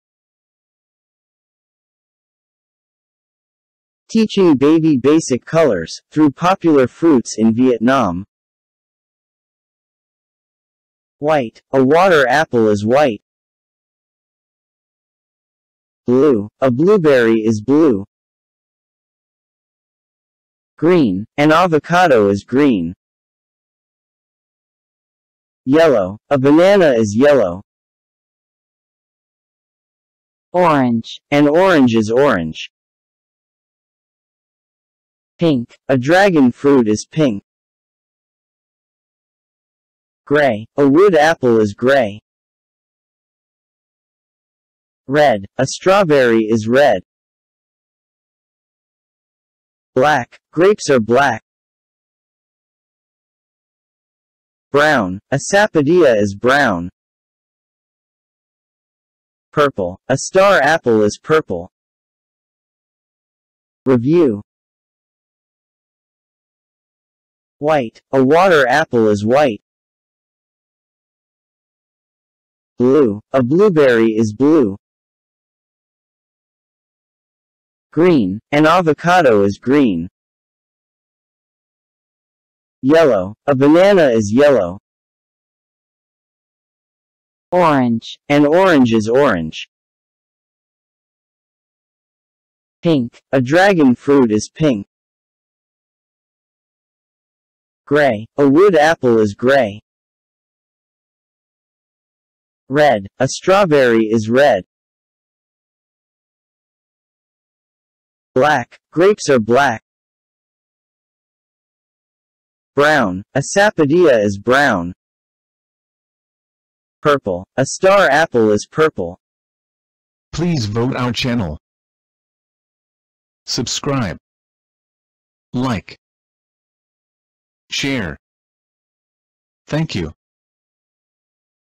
Teaching baby basic colours through popular fruits in Vietnam (English voice)
Teaching baby basic colours (White, Blue, Green, Yellow, Orange, Pink, Grey, Red, Black, Brown, Purple) through popular fruits in Vietnam.